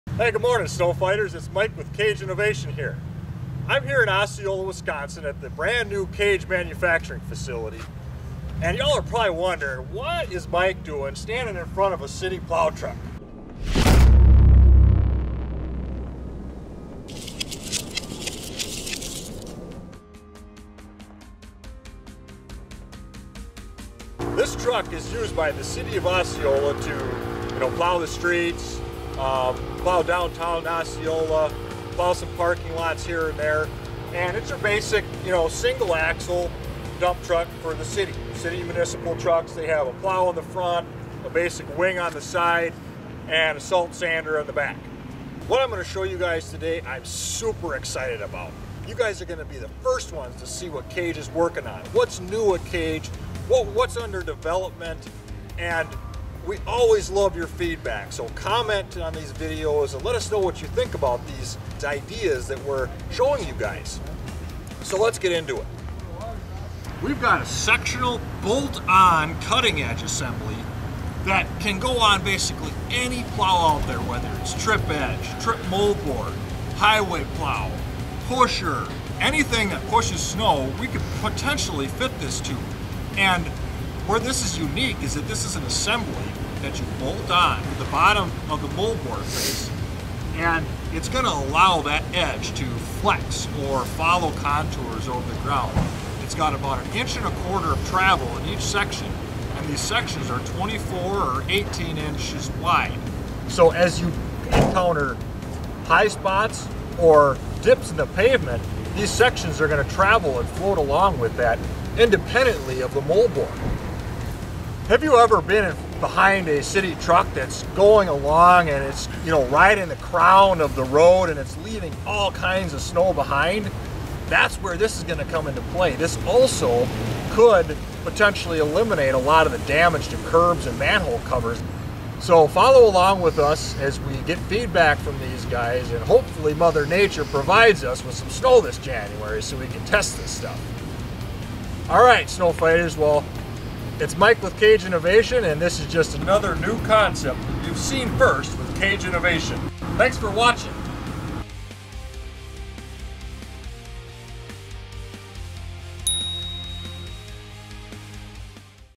Universal Snow Plow Cutting Edge System - AdvantEDGE Research and Development
Kage Innovation's new AdvantEDGE cutting edge system is designed with a universal mounting system, allowing it to not only be used on Kage systems such as the SnowFire, SnowStorm and SnowDozer, but it also can be bolted on to non-Kage snow plows including municipal plows and snow pushers.
Here, we show it in use on a city plow truck as we conduct our research and development for this innovative new product.

0:00 Intro
1:05 Sectional Cutting Edge